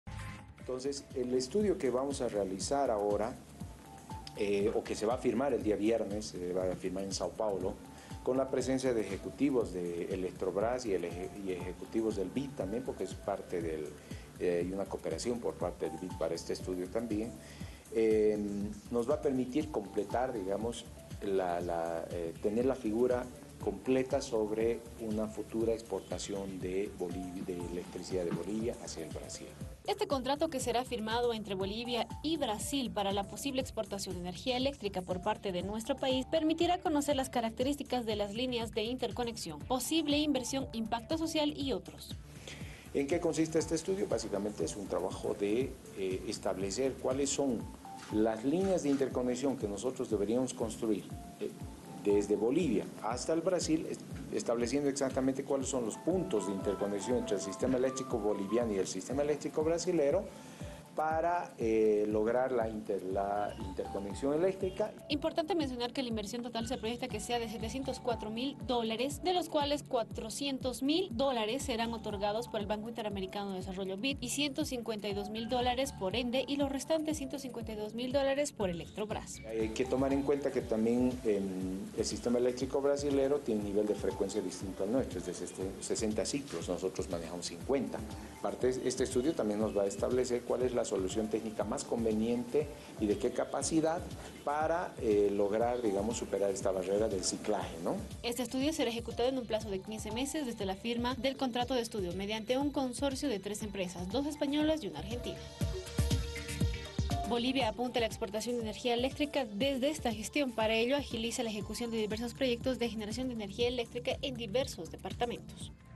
Bolivia y Brasil firmarán acuerdo para el estudio de exportación de energía eléctrica
El presidente de la Empresa Nacional de Electricidad (ENDE) Corporación, Joaquín Rodríguez, anunció que este viernes Bolivia y Brasil firmarán un contrato para el estudio de una futura exportación de energía eléctrica hacia el vecino país.

Asimismo, Rodríguez informó que este estudio (realizará con la cooperación del Banco Interamericano de Desarrollo BID) permitirá establecer las líneas de interconexión que se deberían construir desde Bolivia hasta Brasil,  estableciendo los puntos de entre el sistema eléctrico boliviano y el brasileño para lograr la interconexión eléctrica.